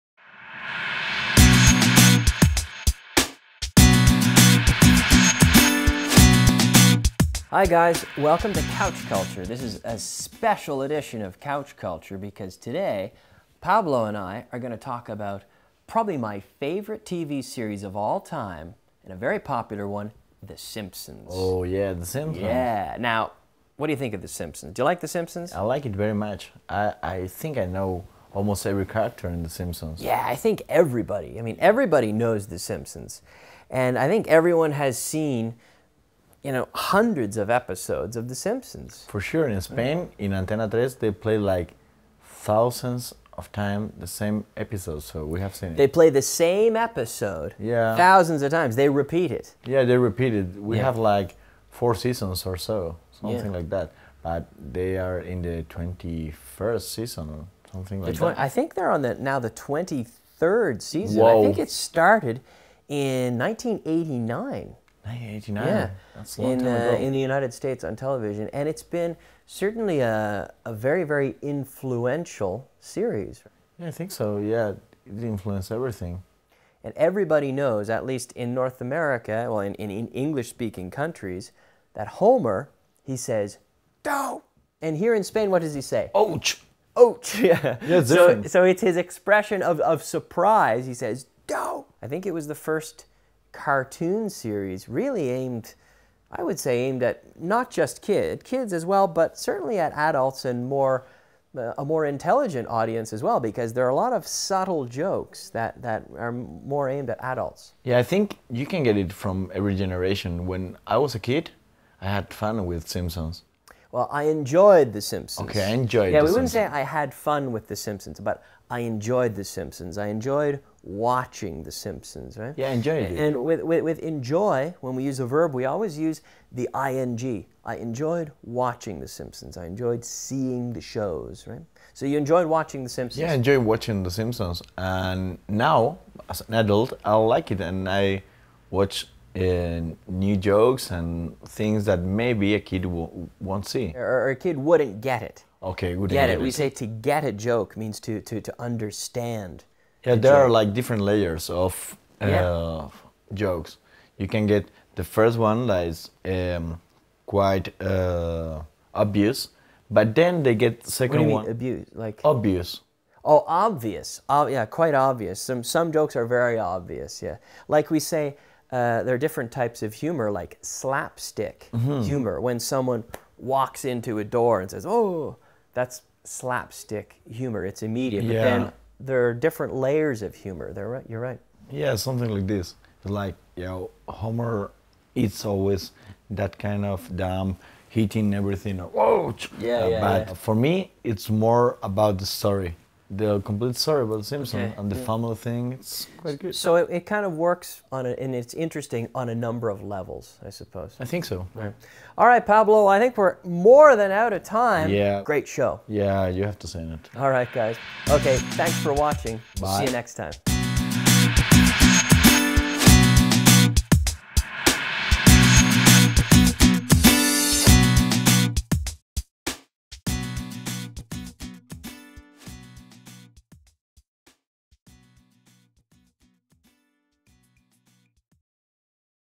Couch Culture #05: The Simpsons
Couch Culture is an educational series dedicated to all things related to entertainment. Join Kyle Millar and Pablo T as they discuss famous movies, tv shows, musicians and more.

Today they are talking about "The Simpsons".

Couch Culture es una serie educativa dedicada al mundo del entretenimiento. Kyle Millar y Pablo T charlan sobre películas, series de TV, música y mucho más.

Hoy hablan sobre "Los Simpsons".